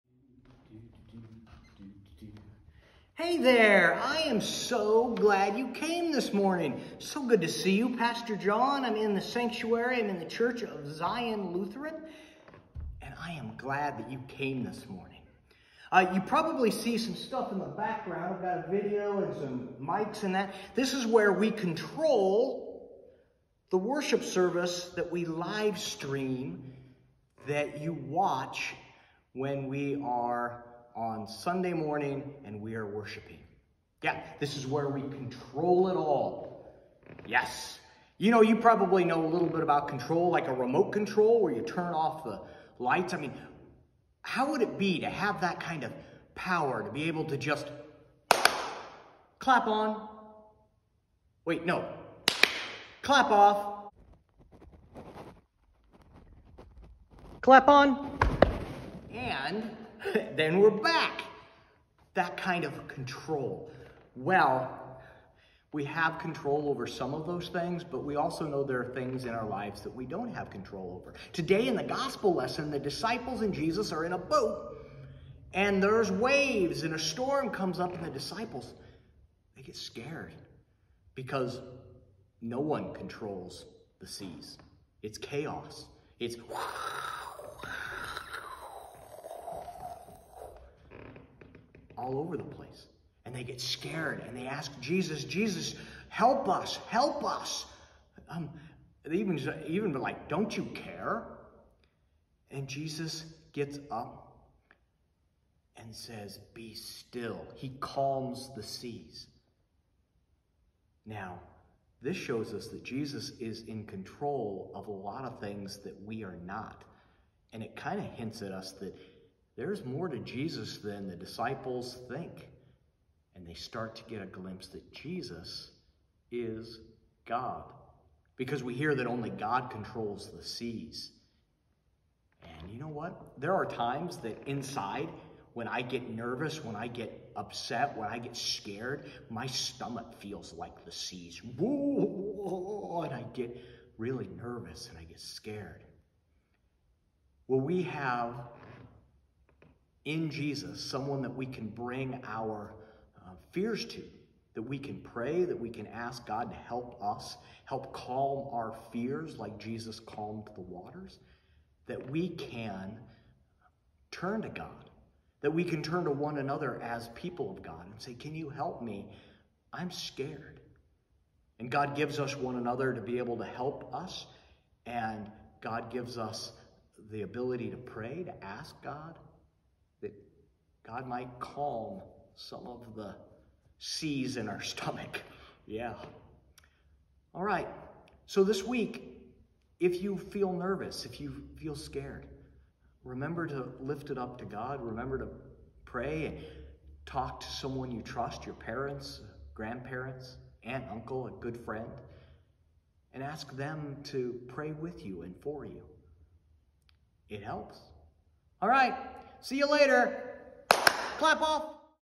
Children’s Sermon for June 20th, 2021
Jesus calms the seas, and we can bring our worries to him.